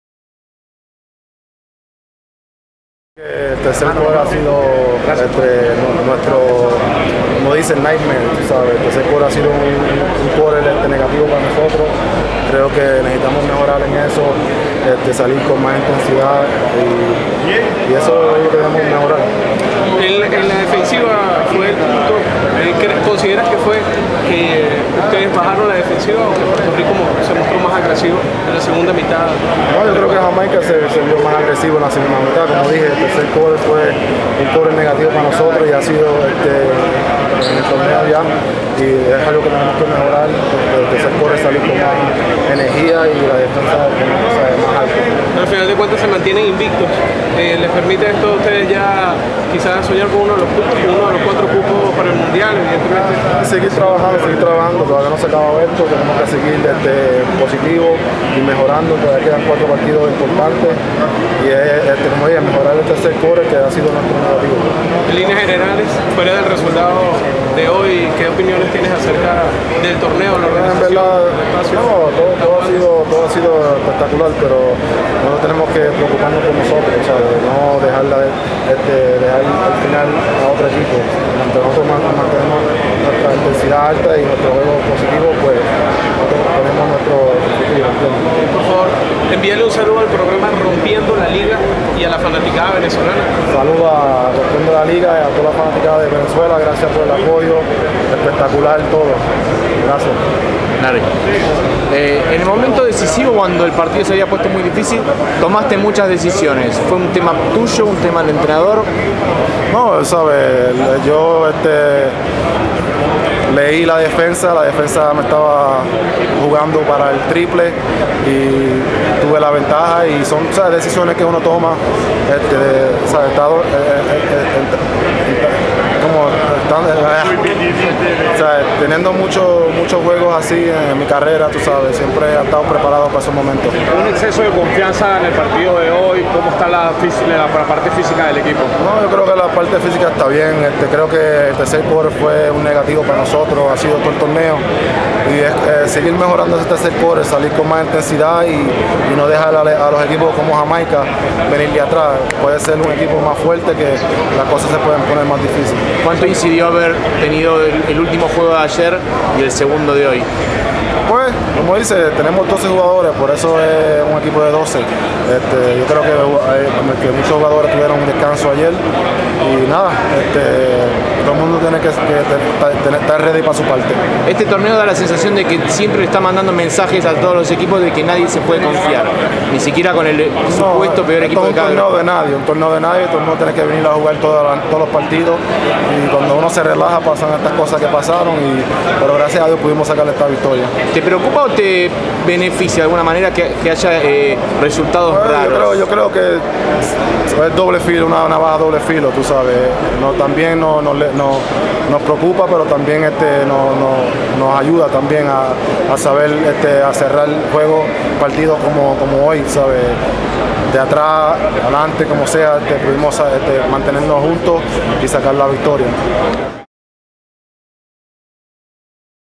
Ayuso: "Tengo muchos partidos así; estoy preparado"
Larry se jugó varios ataques seguidos en el momento de quiebre del partido y dice que está preparado para eso, porque tiene muchos juegos de estos encima.